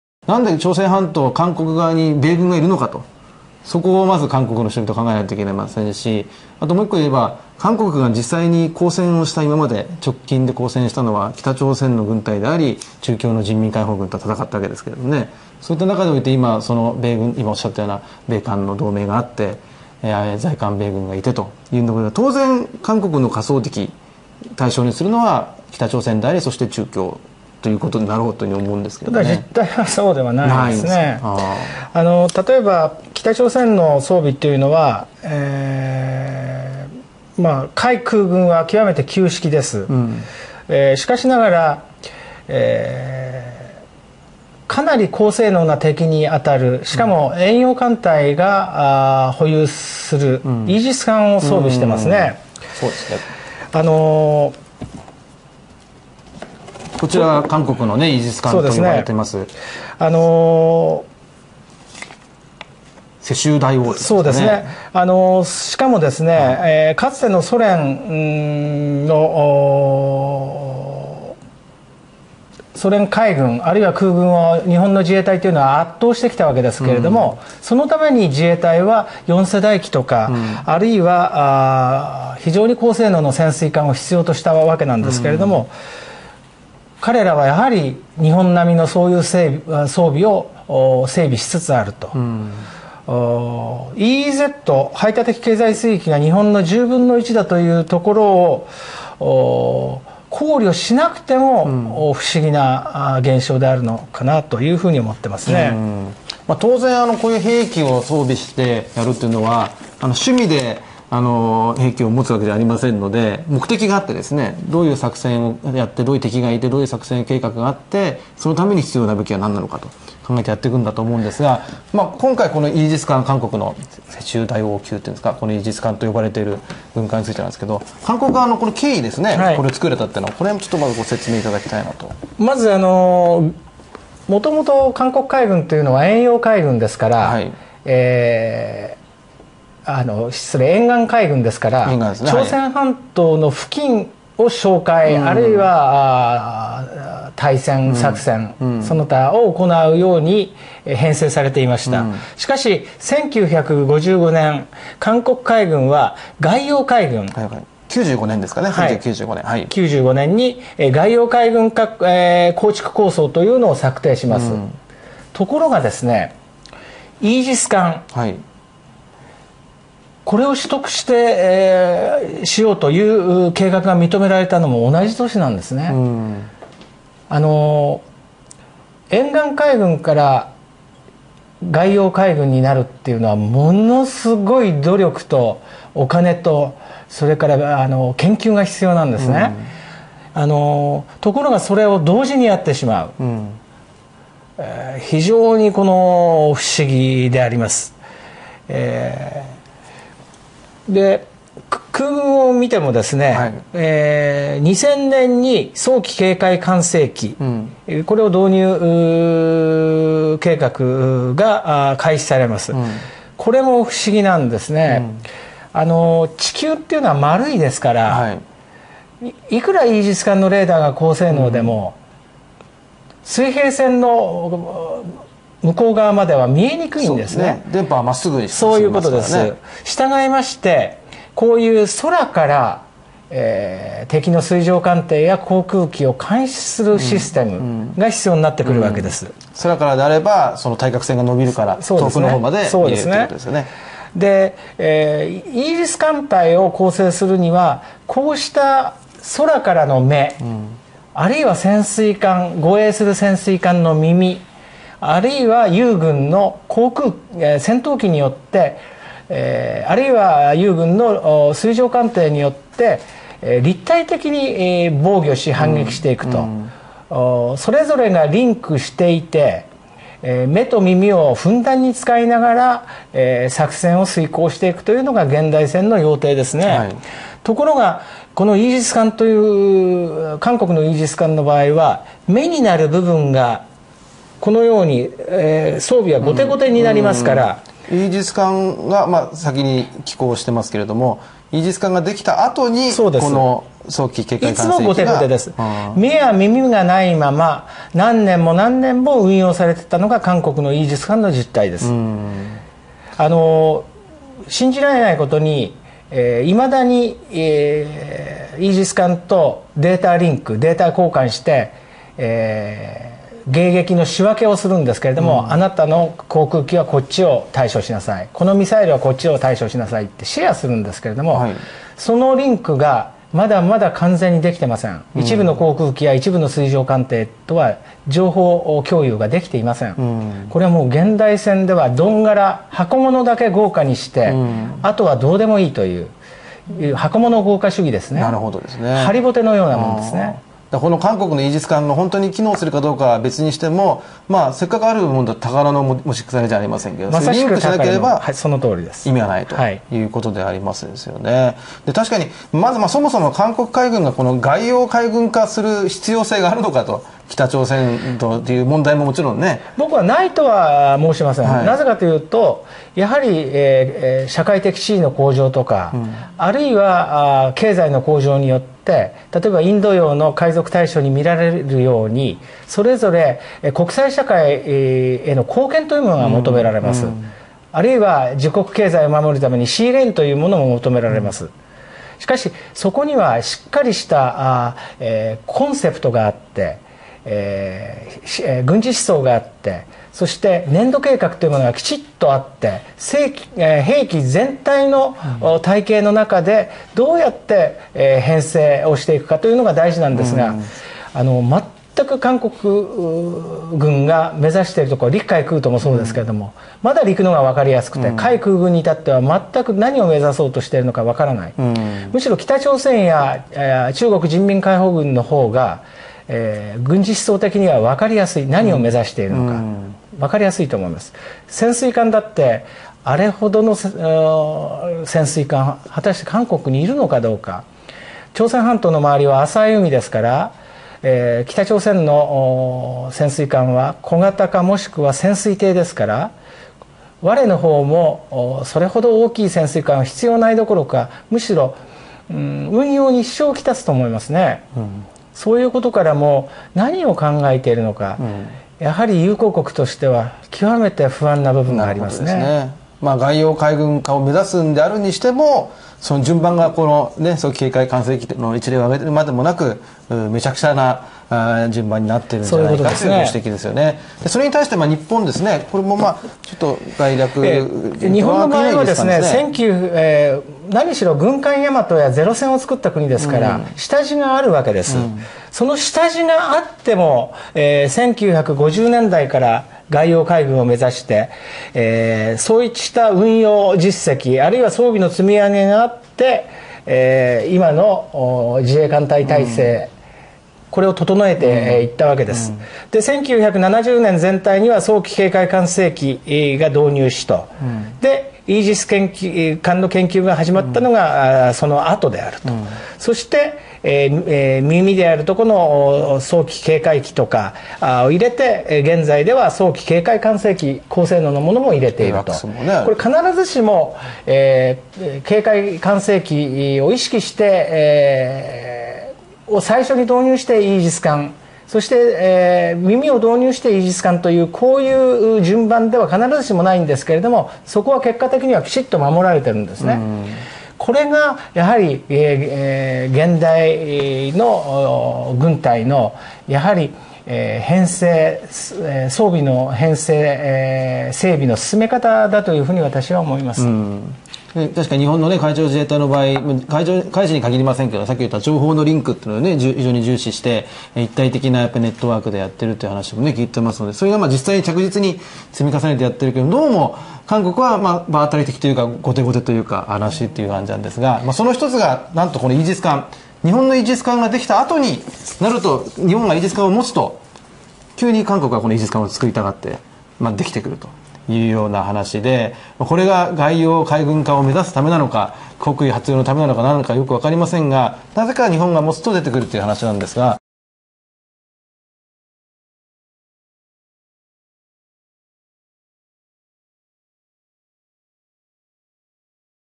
【残念】ハリボテのイージス艦を持つ、北朝鮮・中国軍よりも誇りのない韓国軍
イージス艦に必要な巡回機との連携によるレーダーシステムがない韓国軍のイージス艦はハリボテのようです。
軍隊において必要なコンセプト・綿密な軍事計画どころか、軍事思想すらもない韓国軍は、
北朝鮮や中国軍よりも思想レベルが低いようですね。
イージス艦を手に入れたのも、日本のイージス艦を入手した矢先の出来事だったので、これもお得意のパクリでしょう。
誇りのない軍隊、それが韓国軍。

チャンネル桜 2013年9月27日放送分より